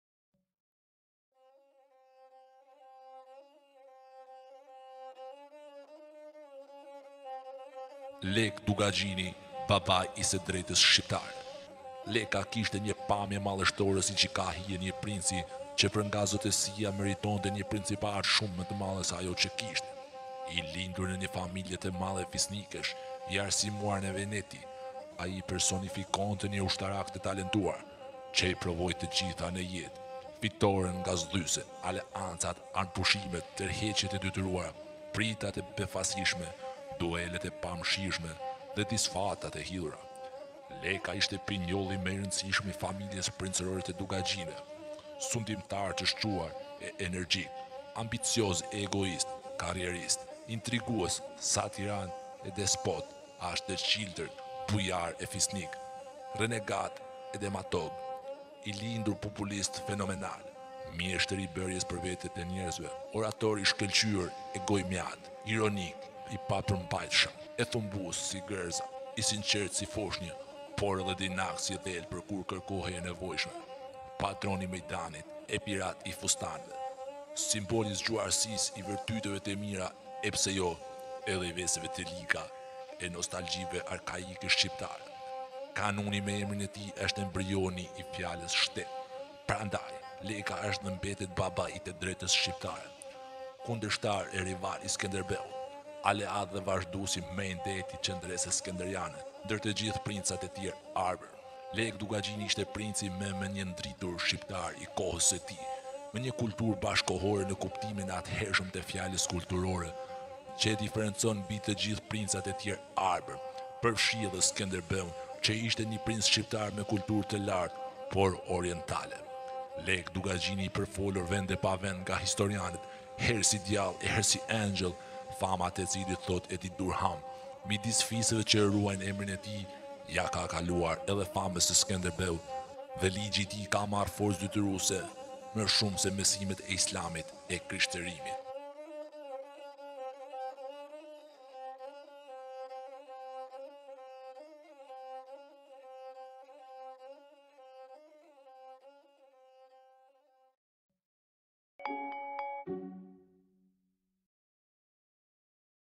Leke Dukagjini dhe kanuni Shqiptar
Lek Dukagjini ishte i pari qe vendosi kushtetuten Shqiptare te kohes "Kanuni"

Muzika dhe orkestrimi: A.P

By D.Y